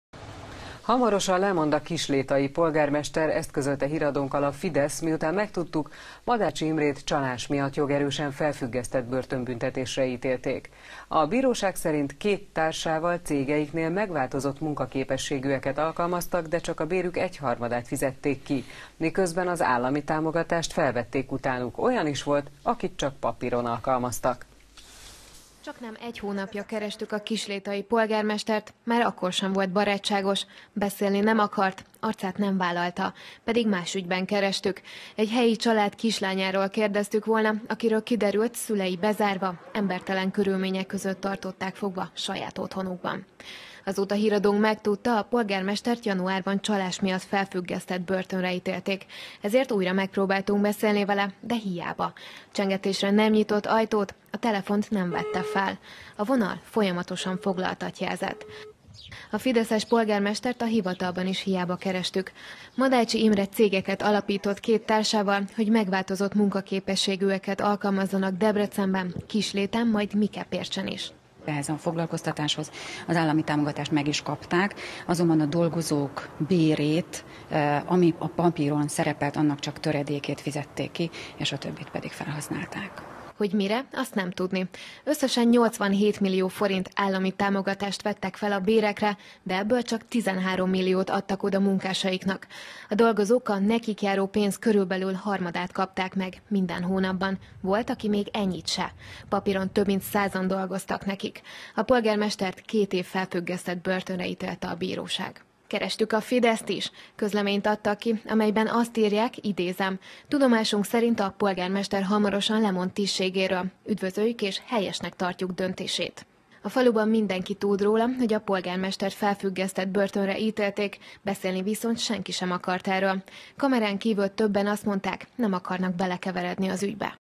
Hamarosan lemond a polgármester 15-05-25
Hamarosan lemond a kislétai polgármester – ezt közölte híradónkkal a Fidesz, miután megtudtuk: Madácsi Imrét csalás miatt jogerősen felfüggesztett börtönbüntetésre ítélték. A bíróság szerint két társával cégeiknél megváltozott munkaképességűeket alkalmaztak, de csak a bérük egyharmadát fizették ki, miközben az állami támogatást felvették utánuk. Olyan is volt, akit csak papíron alkalmaztak.